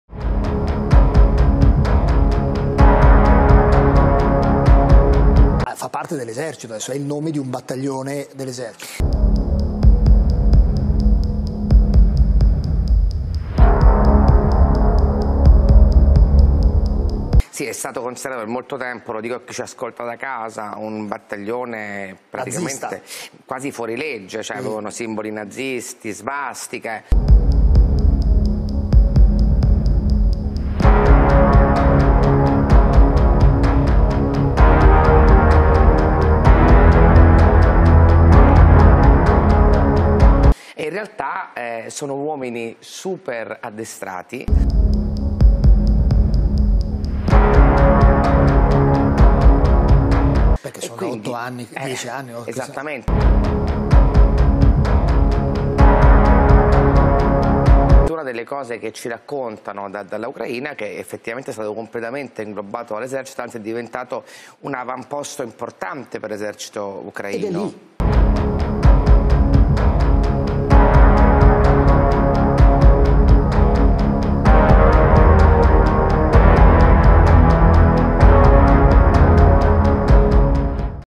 Myrta Merlino e il Battaglione Azov
L'incredibile sdoganamento del neo-nazismo in Italia sulle tv italiane prosegue. Ma in questo minuto della Merlino si è sicuramente toccato l'apice.